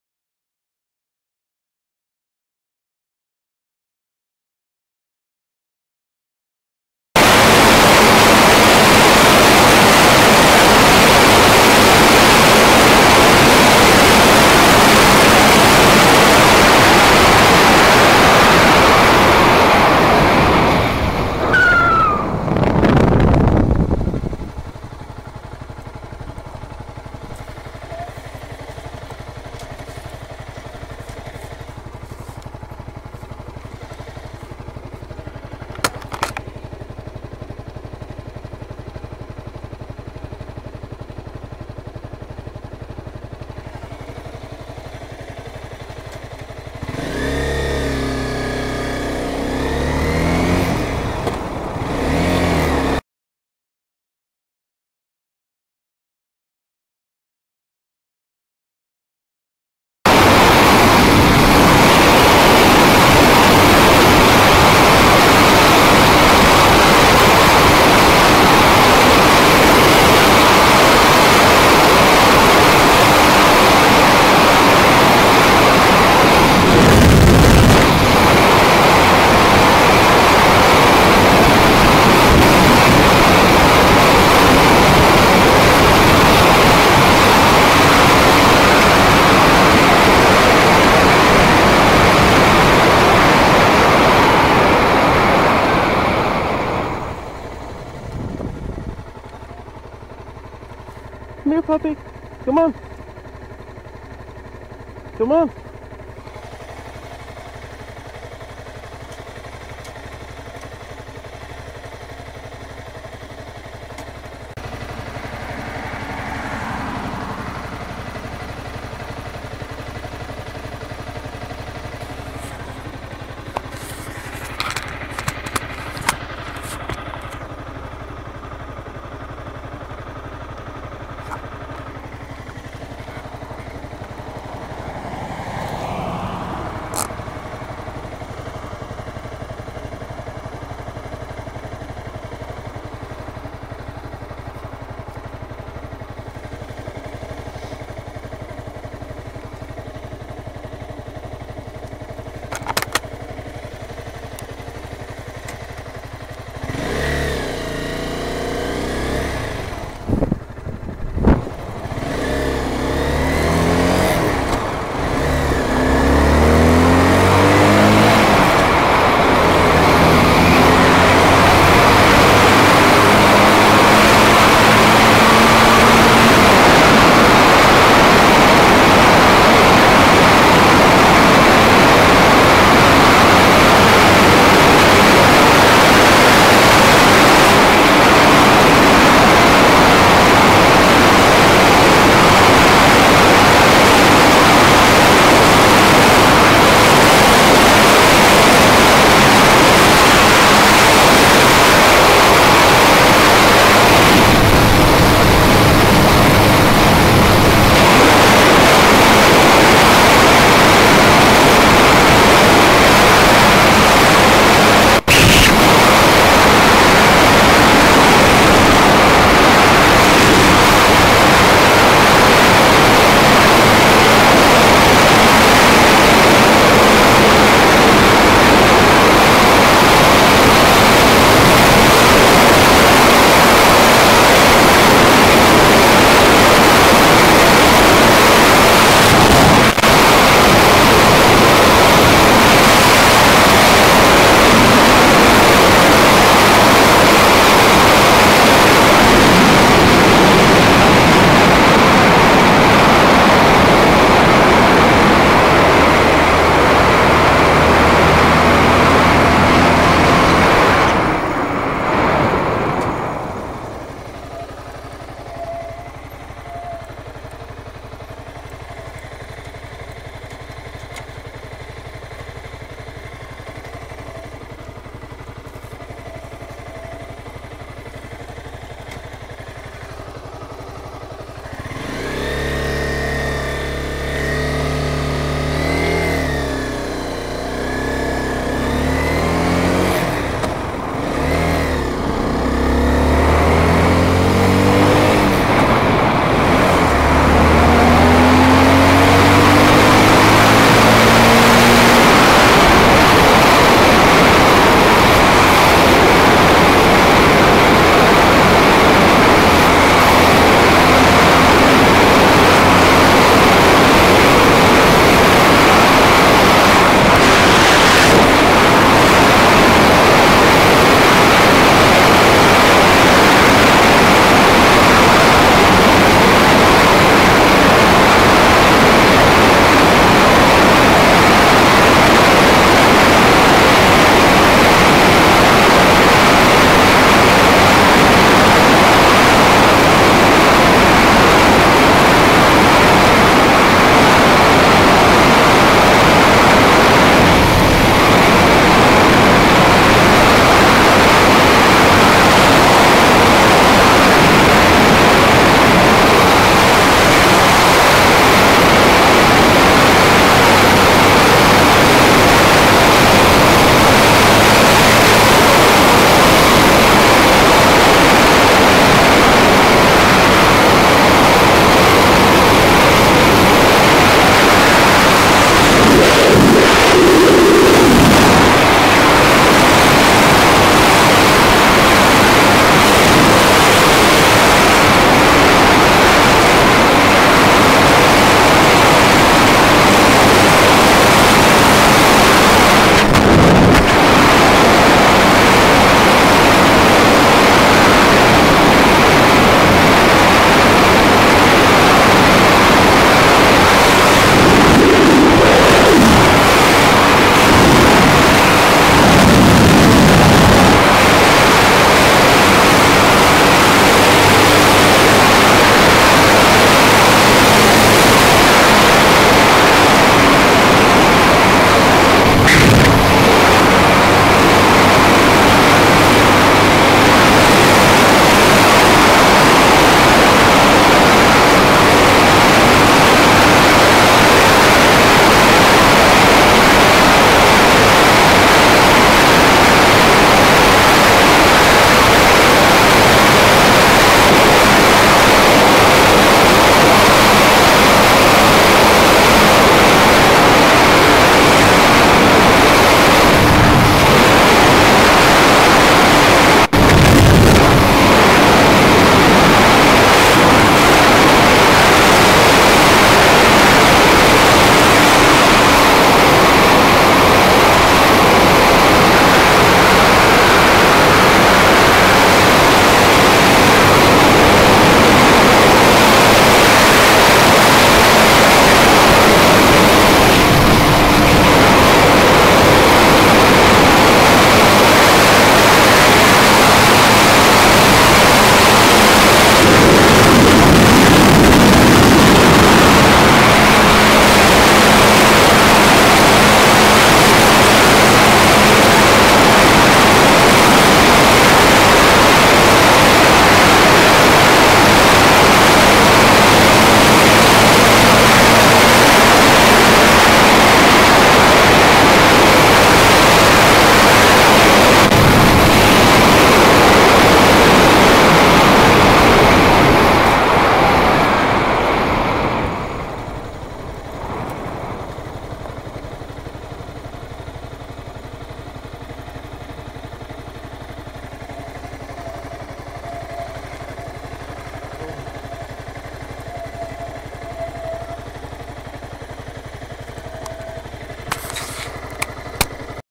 Wildlife on the Natchez trace
Spotting a deer. Coming across some stray dogs, and a night drive.